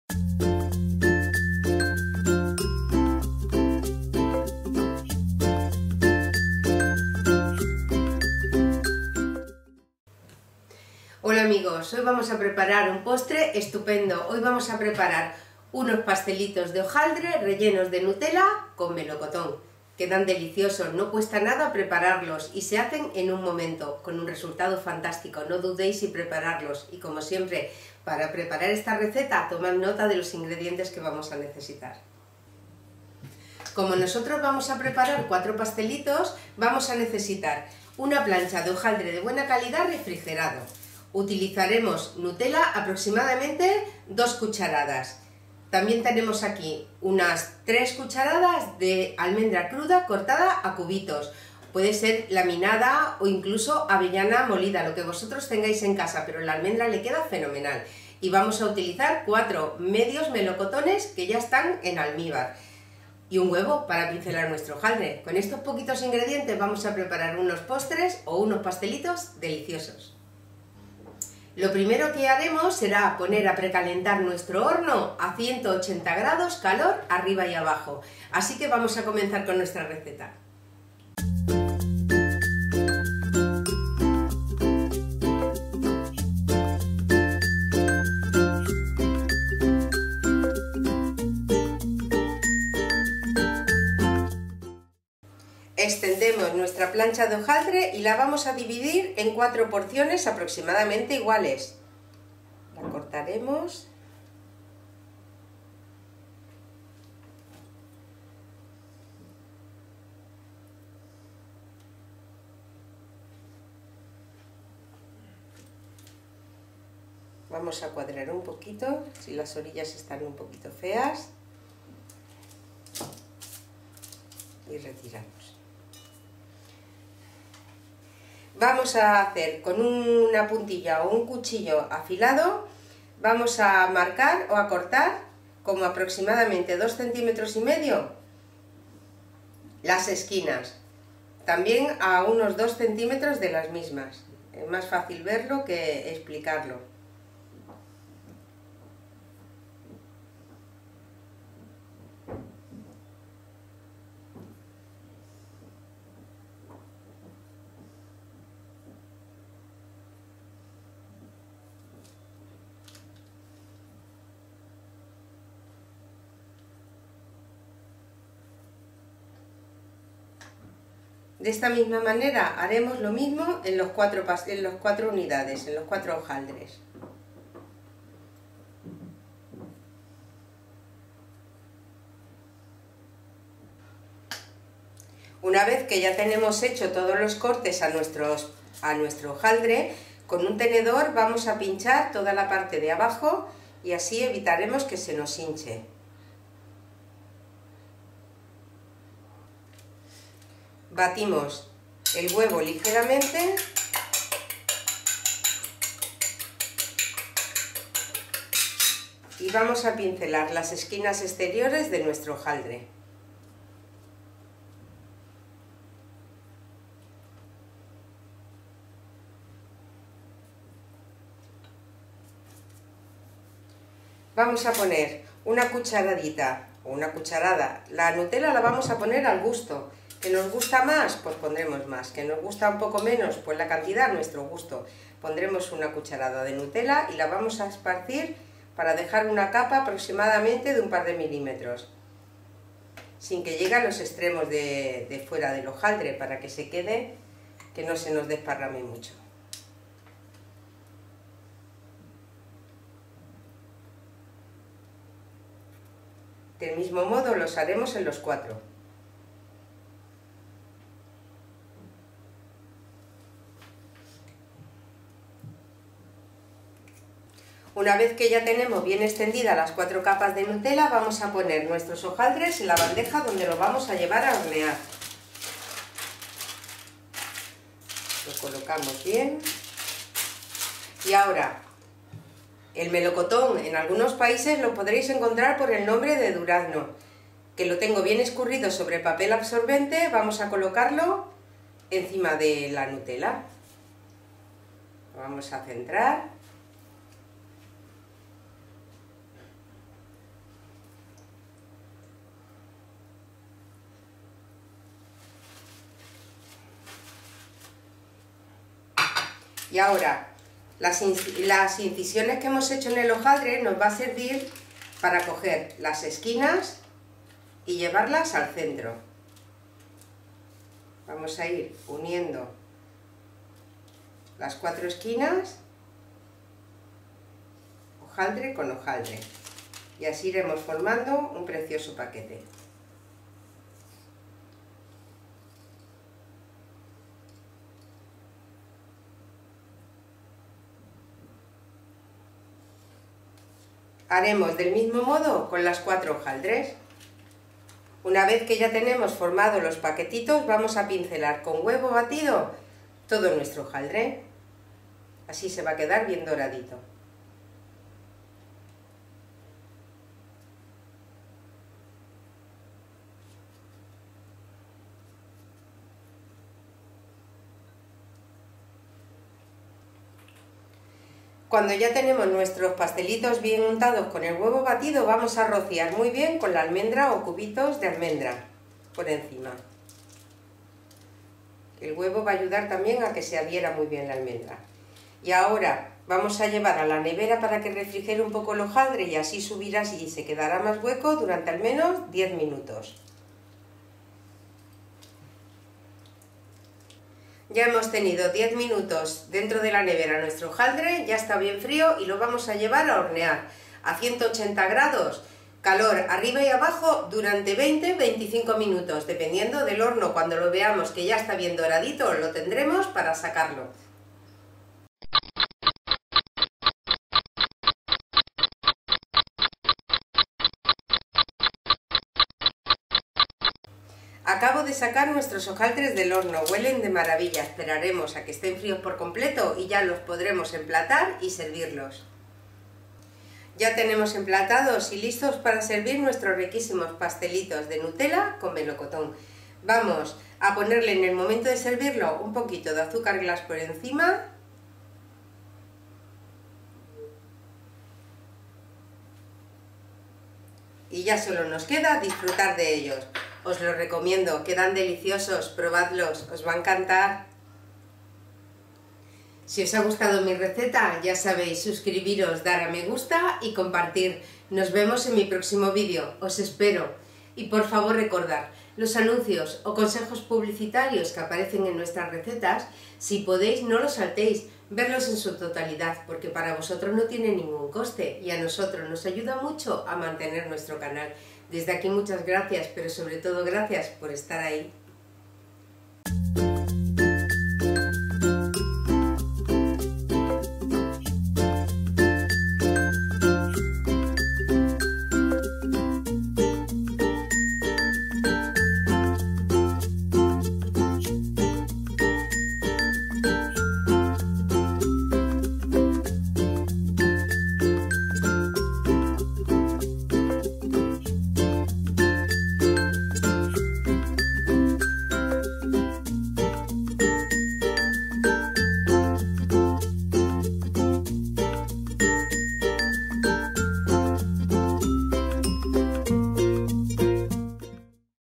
Receta Hojaldre relleno de Nutella y melocotón - Recetas de cocina, paso a paso. Loli Domínguez
Hoy vamos a preparar un postre estupendo, vamos a preparar unos pastelitos de ¡Hojaldre relleno de Nutella y melocotón! Quedan deliciosos, no cuesta nada el prepararlos y se hacen en un momento con un resultado fantástico, no mudéis y preparadlos y como siempre os digo prepararlos os encantara.
Ingredientes:
1 Plancha de hojaldre refrigerada
Al gusto Nutella
Al gusto Almendra cruda en tracitos (Laminada también valdría)
4 Medios melocotones en almíbar
1 Huevo (Para pincelar el hojaldre)
Al gusto azúcar glass